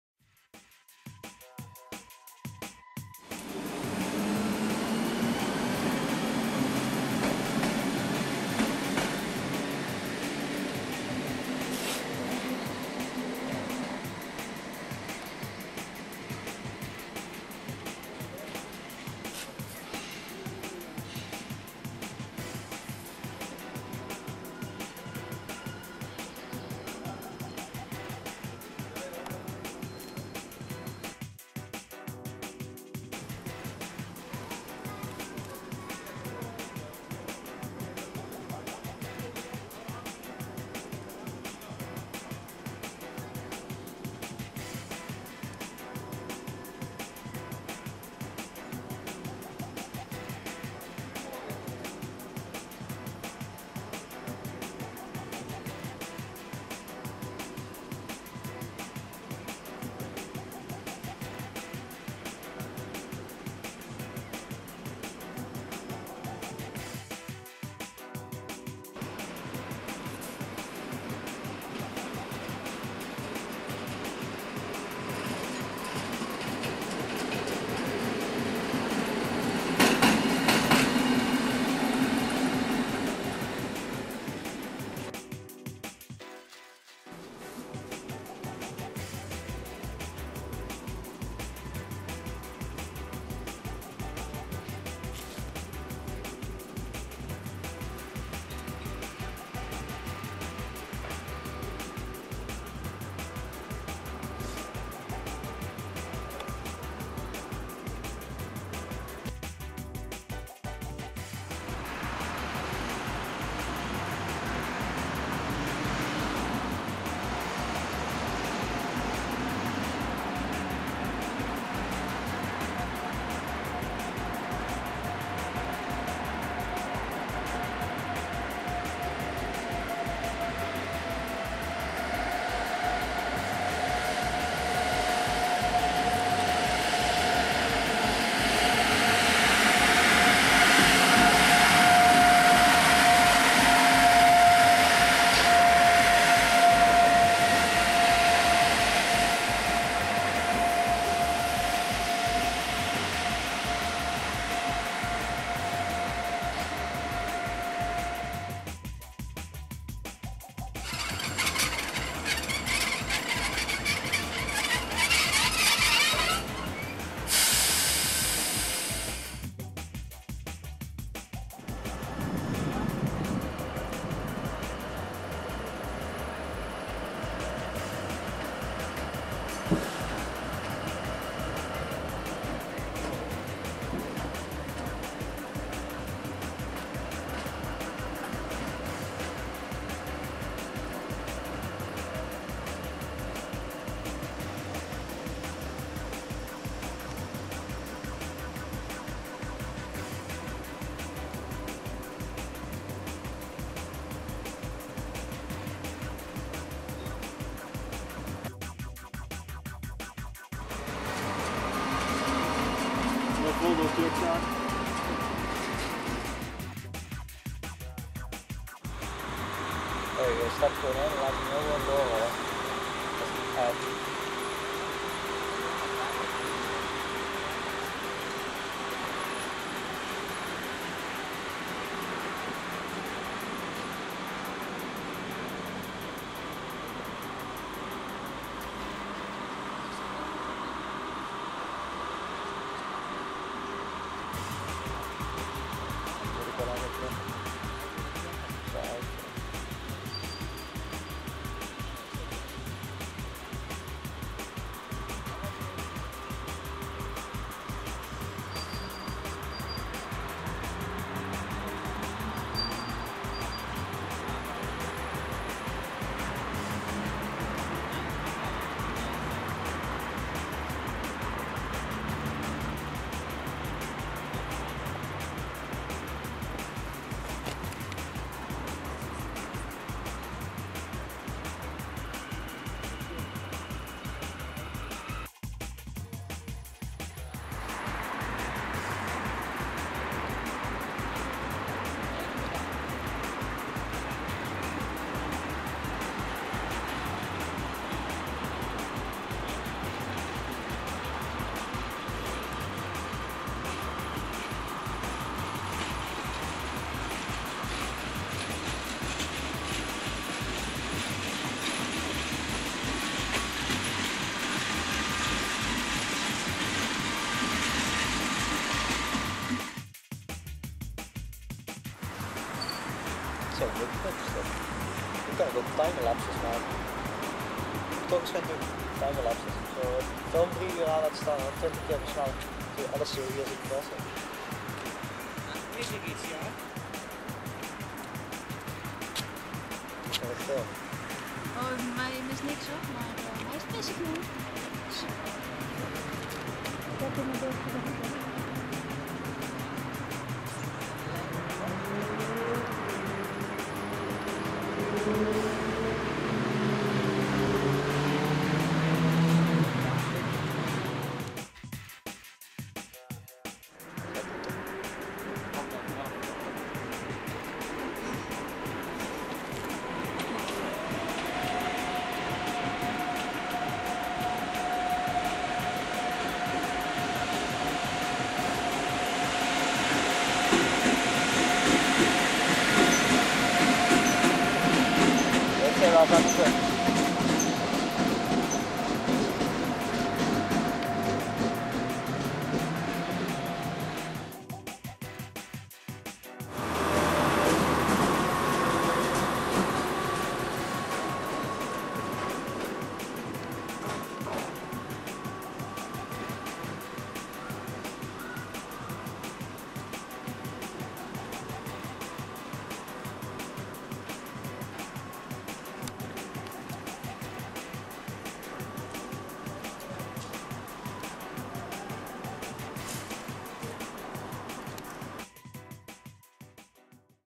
01-05-09 | Venlo | Shunterzzz
Een hoop fragmenten van shunters en geshunt op Venlo